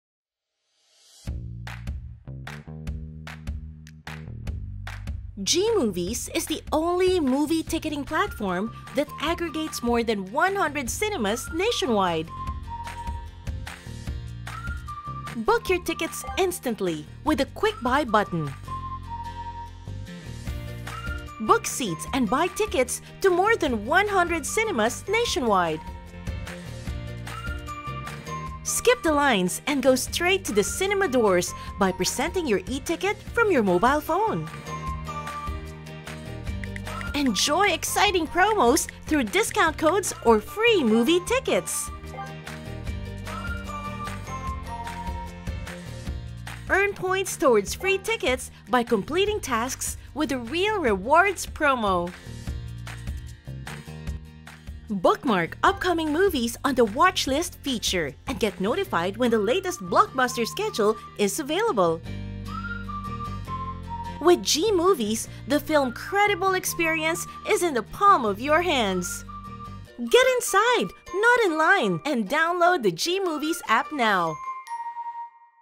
GMovies - App Features
The ultimate cinema experience is now in the palm of your hands! Check out the movie schedules for cinemas nationwide, reserve seats in advance, choose from multiple payment options and get the latest news and movie reviews.

Book seats, buy tickets, and say goodbye to long cinema lines with the GMovies app!